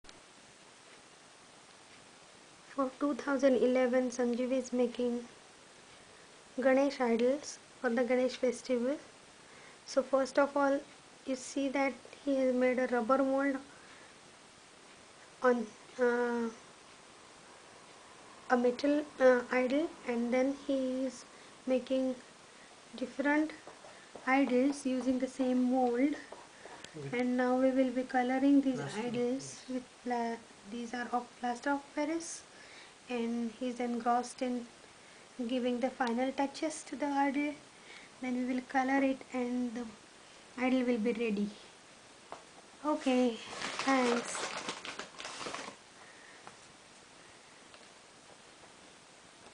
Ganapati Idol Frankfurt 2011-Sanjeev Joshi
Sanjeev Joshi has been preparing Ganesh Idols at home for last 12 years. He is preparing idols with clay by hand. This year -2011, in order to makemore idols for some of the family friends he prepared first a mould at home and then cast the idols with plaster. This video shows finsihing of Ganseh Idol made from a self made mould.
Learned this art  from Mr. Rahukar , Pune (murtikar Rahukar).